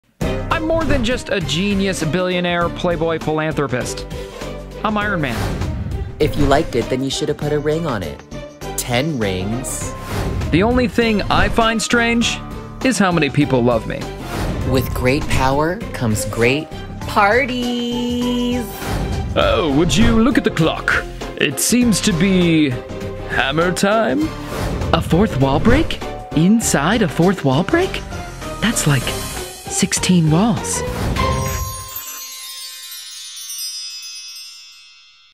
if the avengers had a reality show
coming to mcu phase 37 on disney+

@KurtTocci as tony stark, dr. strange, thor, and deadpool

me as shang-chi, spider-man obvi